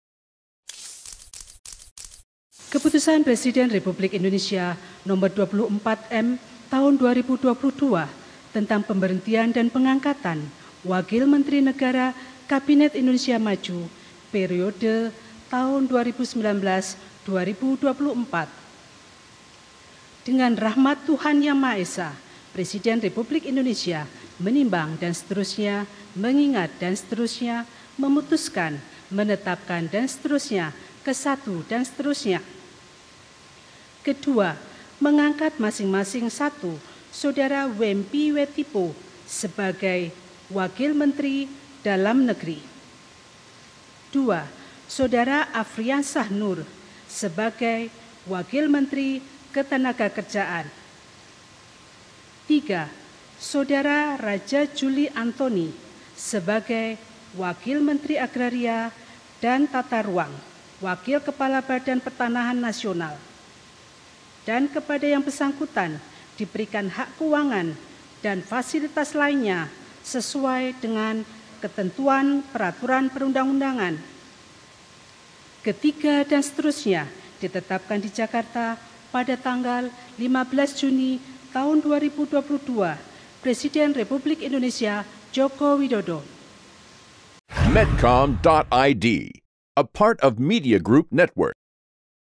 Reshuffle Kabinet, Jokowi Lantik 3 Wakil Menteri Baru
Presiden Joko Widodo melantik tiga wakil menteri (wamen) baru di Kabinet Indonesia Maju. Pelantikan digelar di Istana Negara, Jakarta, Rabu (15/6/2022).

Pelantikan ketiga wakil menteri berdasarkan pada Keputusan Presiden Nomor 24/M Tahun 2022 tentang Pemberhentian dan Pengangkatan Menteri Negara Kabinet Indonesia Maju Periode Tahun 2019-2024.

Wamen yang dilantik adalah Wamen ATR/BPN Raja Juli Antoni, Wamendagri John Wempi Watipo, dan Wamenaker Afriansyah Noor.